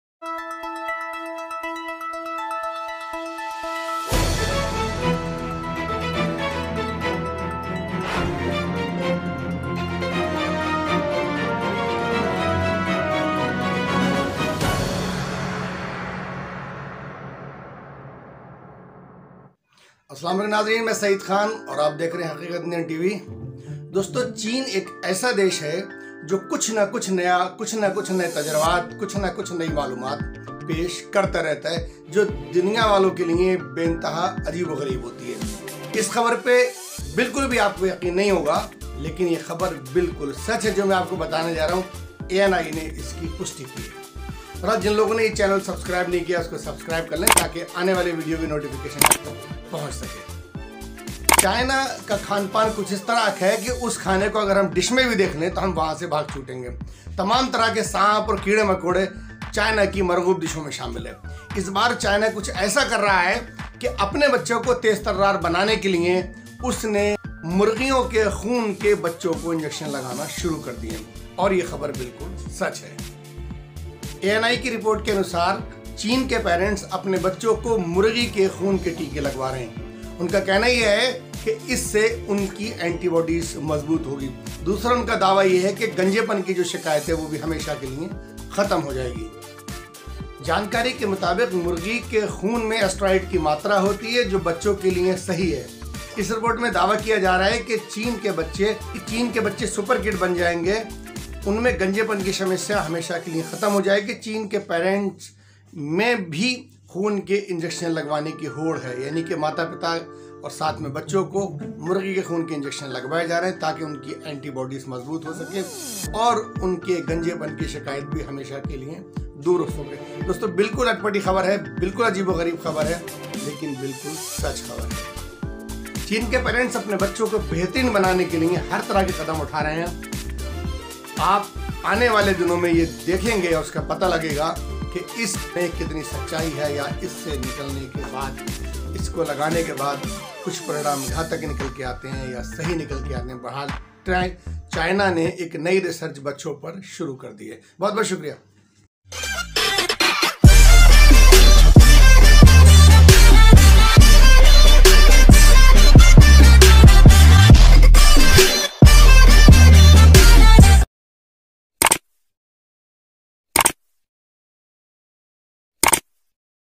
सुपरकिड बनाने के लिए बच्चों को मुर्गे के खून का इंजेक्शन लगवा रहे हैं चीन के पैरेंट्स, 16Septe 20r21
सुपरकिड बनाने के लिए बच्चों को मुर्गे के खून का इंजेक्शन लगवा रहे हैं चीन के पैरेंट्स, आपका क्या ख़्याल है?
haqeeqat indian tv haqeeqatindiantv haqeeqat indian tv
चीन (China) एक ऐसा देश है, जो कुछ अलग करने के चक्कर में रहता है. दुनिया (World) को टक्कर देने के लिए वो कुछ ऐसा करता है, जो बेहद विचित्र (Weird) होता है. खाने के मामले में चीन दुनिया से सबसे अलग है. कीड़ा मकोड़ा, ज़हरीला सांप से लेकर चीन के लोग वो सबकुछ खा सकते हैं, जिसकी हम कल्पना तक नहीं कर सकते हैं. अभी हाल ही में एक ख़बर सोशल मीडिया पर तेज़ी से वायरल हो रही है. इस ख़बर के मुताबिक, चीन के लोग अपने बच्चों के शरीर में मुर्गे के ख़ून का इंजेक्शन ('Chicken blood injections', the 'cure-all' therapy for Chinese kids) लगवा रहे हैं. ये थोड़ा अटपटा तो है, मगर सच है.